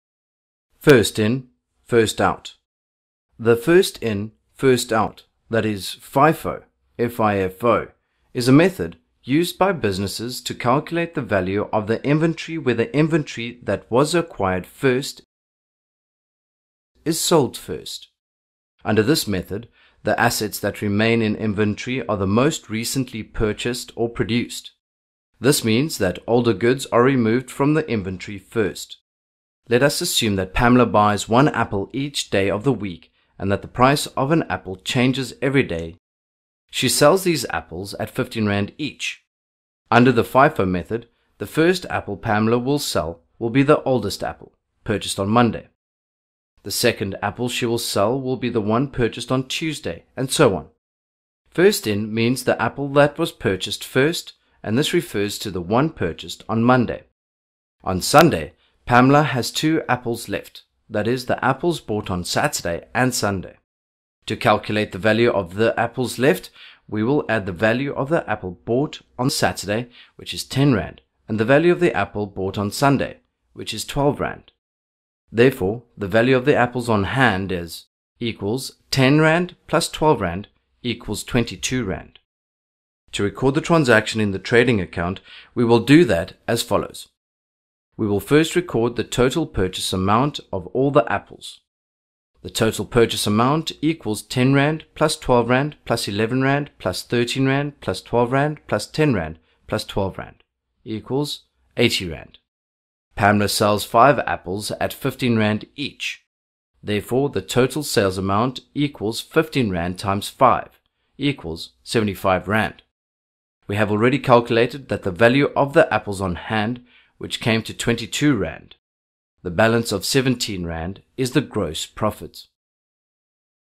FIFO method
Grade 12 Accounting:
The first-in-first-out (FIFO) is a method used by businesses to calculate the value of the inventory where the inventory that was acquired first is sold first.